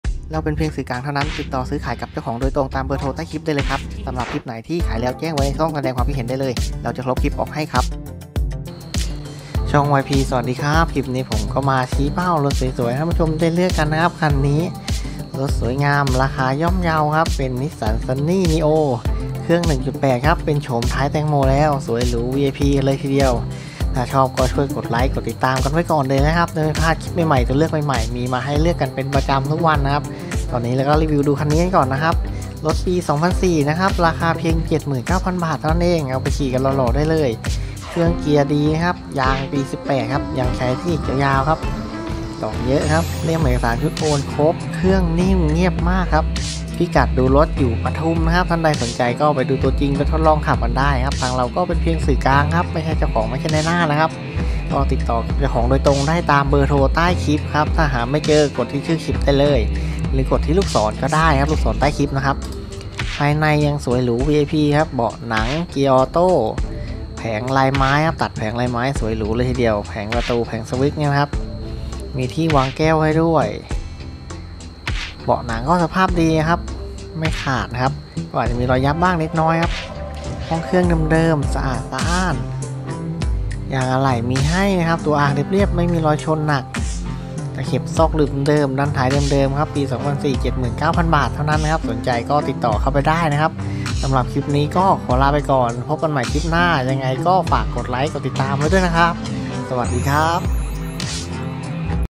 รถสวยสไตล์ vip ราคาย่อมเยา💥 พร้อมใช้งานได้เลย
2004 Nissan Sunny

฿79,000

2004 Nissan Sunny · Sedan · ขับไปแล้ว 230,000 กิโลเมตร

ขายสด 79,000 เท่านั้นครับ

ปี 2004 Nissan sunny neo 1.8 (ไฟท้ายเม็ดแตง)
เครื่องเกียร์ดี
ยางปี18
เอกสารพร้อมโอน
เครื่องนิ่งเงียบมาก
รถอยู่ปทุมธานี